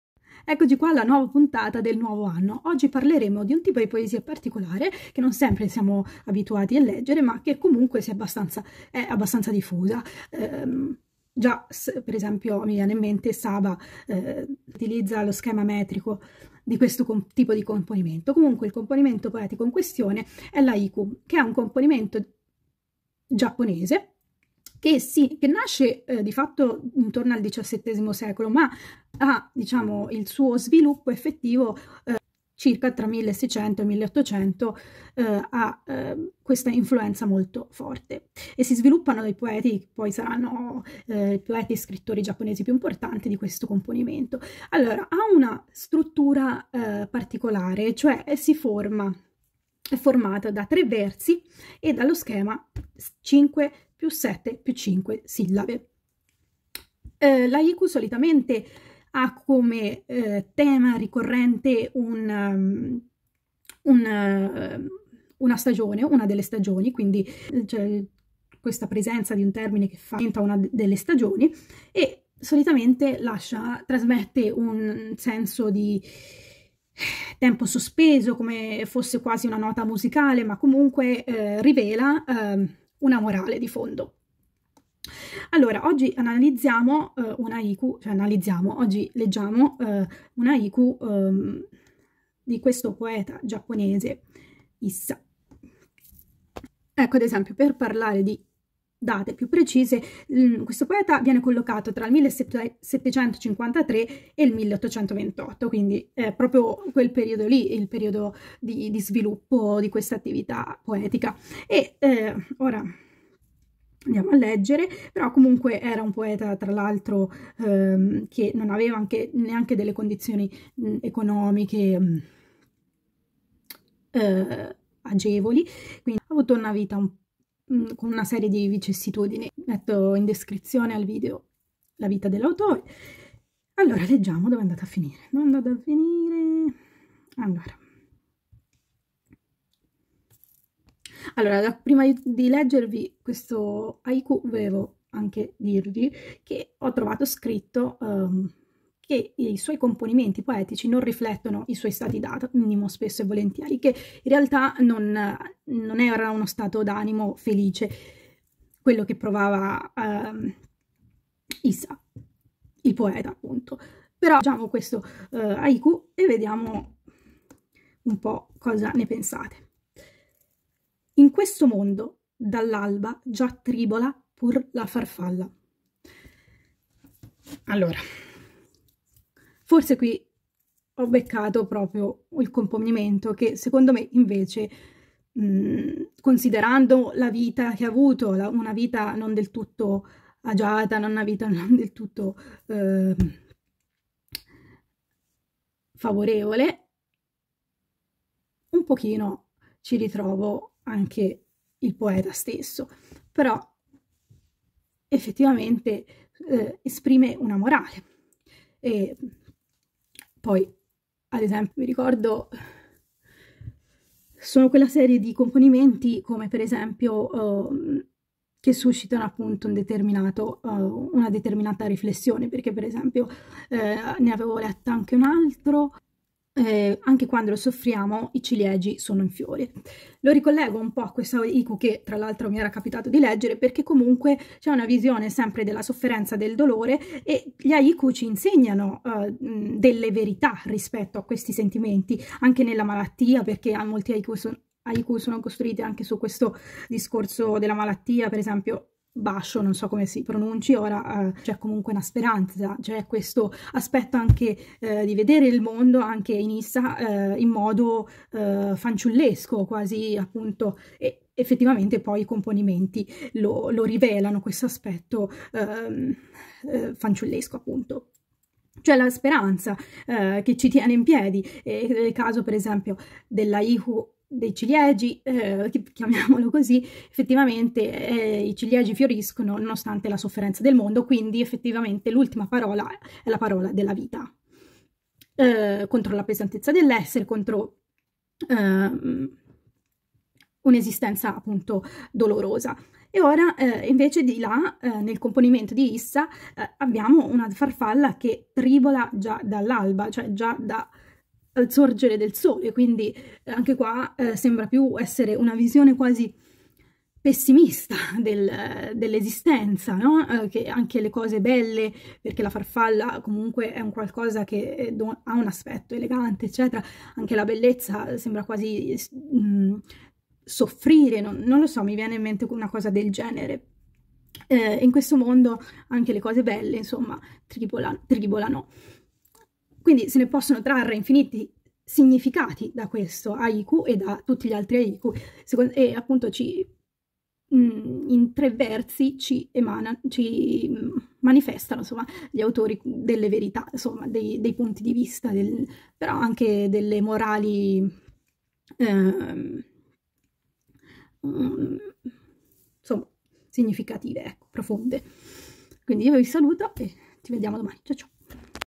Leggiamo una poesia di Kobayashi Issa
nasce da una famiglia di agricoltori. Resta orfano di madre a tre anni, e viene cresciuto dalla nonna che lo lascerà a 14 anni.  Anni dopo il padre si risposa, e  più tardi nasce il suo fratellastro. Si sposa tre volte (e gli muoiono vari figli). Nel 1828 un incendio distrugge la sua casa; Issa muore pochi mesi dopo, senza neppure avere il tempo di vedere la bambina che sua moglie porta in grembo.

scusate stasera non riuscivo a parlare bene la mia lingua  😅😂.

Ps: 5+7+5 = significa un verso quinario, un settenario e un altro quinario